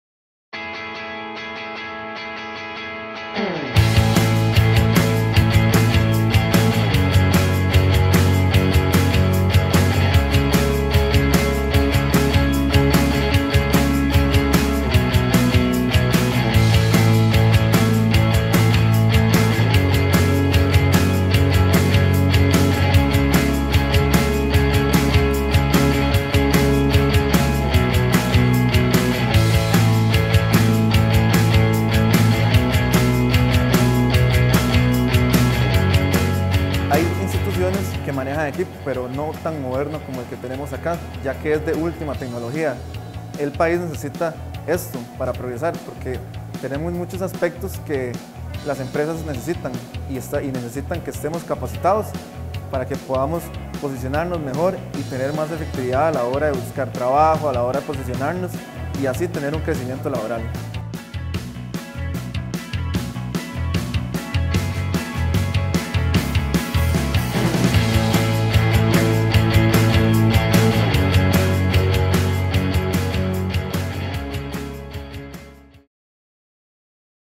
INA sede Heredia - Nuevo laboratorio de Manufactura
Es el único laboratorio de Manufactura Integrada por Computadora en Costa Rica. Está en la sede del INA de Heredia. Simula un proceso real de producción masivo.